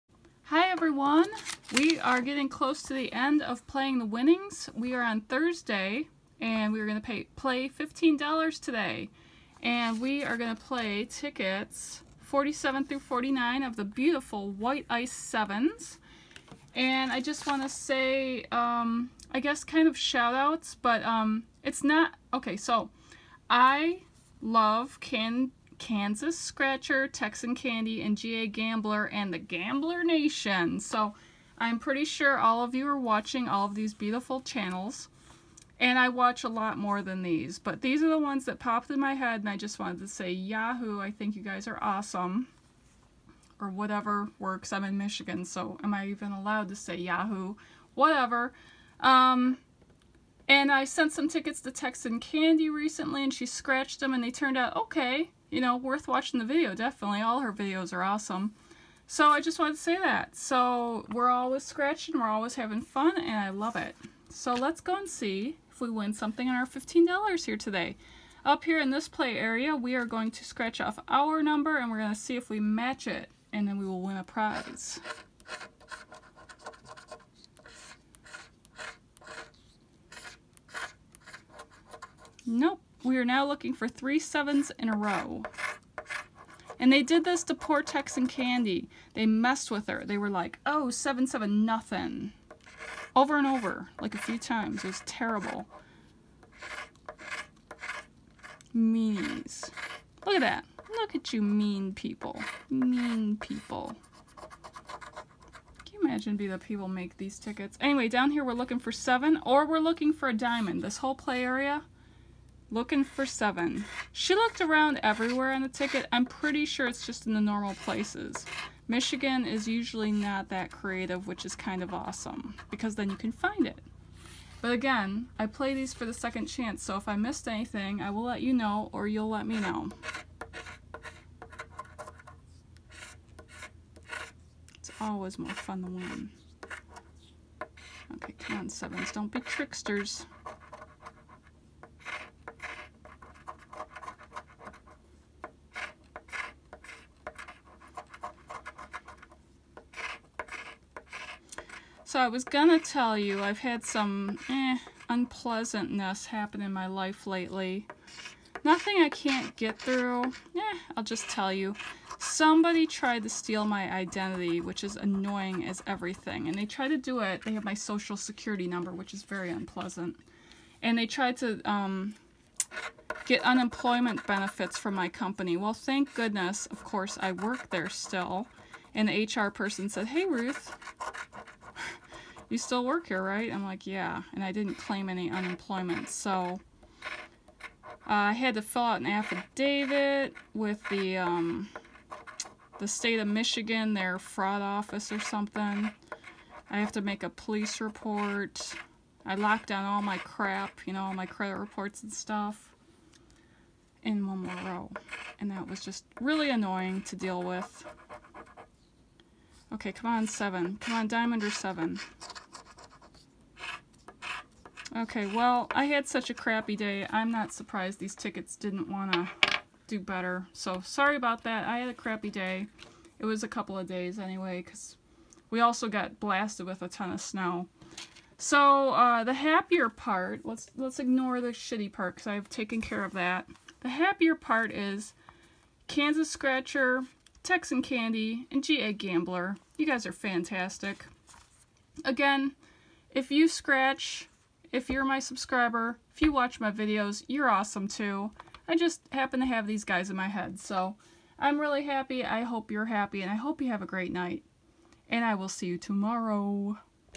Playing the Winnings! Part 12 - 12/14/17
Playing the winnings from the winners from the pack! Let's find some good prizes!